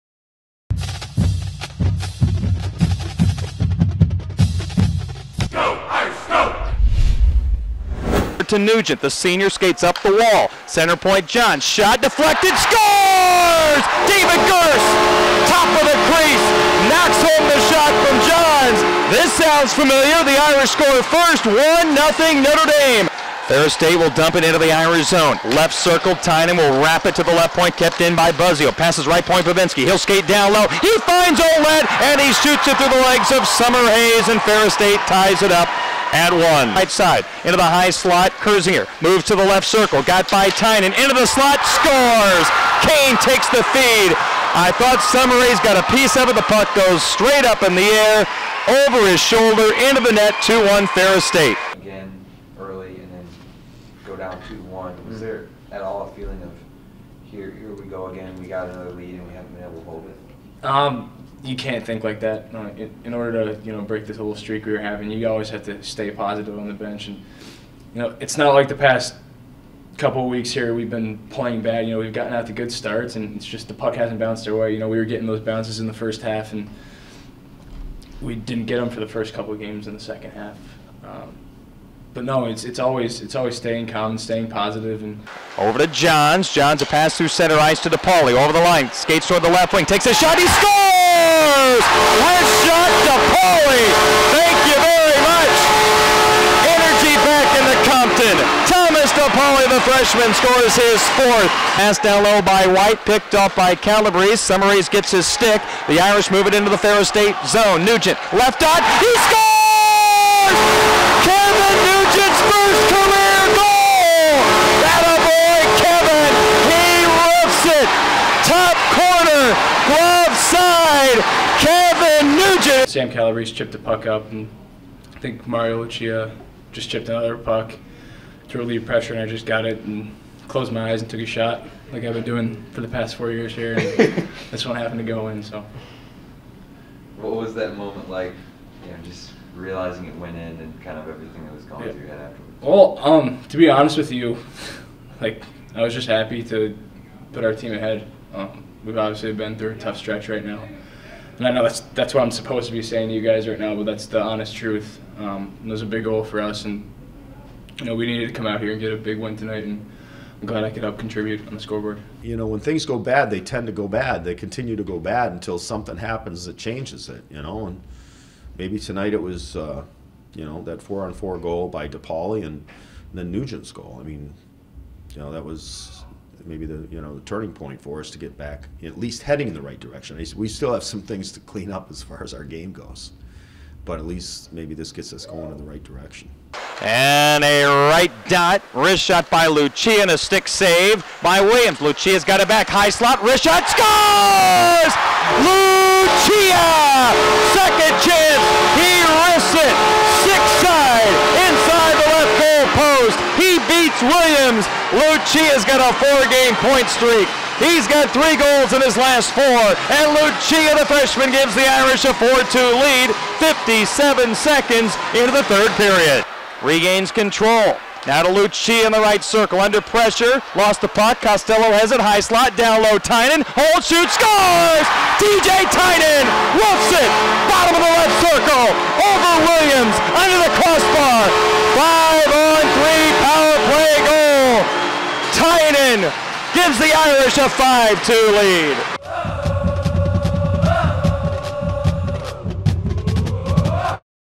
Irish Defeat Ferris State 5-2 - Hockey
Notre Dame split the weekend series with the Ferris State Bulldogs with a 5-2 victory Saturday night at the Compton Family Ice Arena.